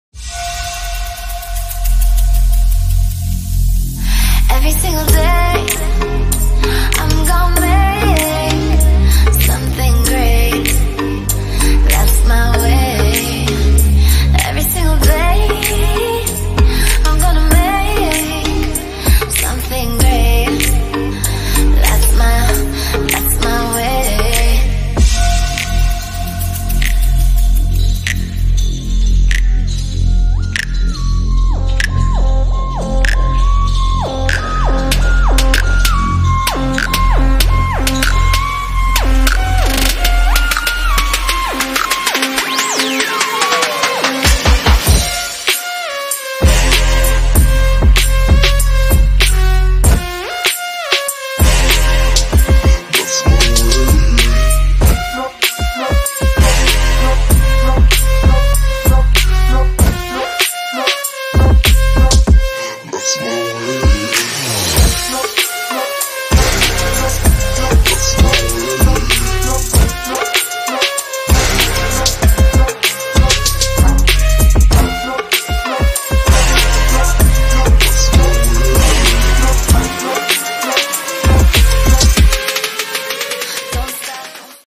Best of Croatia Island Mljet
Mljet one of Croatia most beautiful island, eery interesting, wild and authentic island. Discovering small paradisiacal islands, the most beautiful sunsets, sea and beautiful beaches...